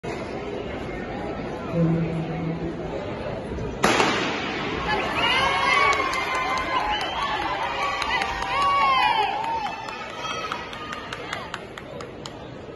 2023 BSC Icebreaker Women 60m
Trinity Provost runs 8.18 in the 60m preliminary